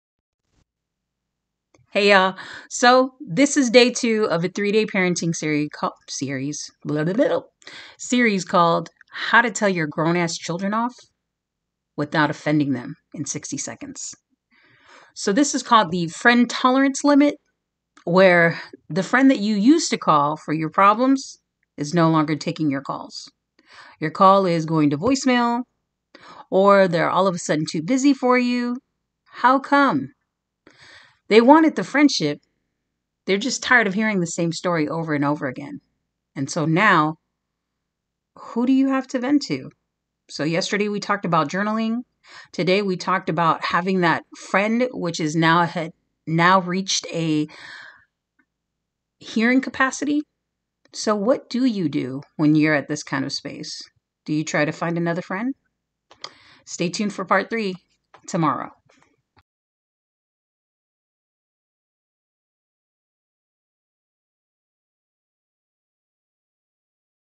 HOW TO TELL YOUR GROWN ASS KIDS OFF IN 60 MINUTES Part 2 of 3 FRIEND TOLERANCE LIMIT FRAN FOMAI
Have you been calling a friend that is dodging you? Are you wondering what's wrong? Have you been telling her the same thing over and over again? There's your answer friend.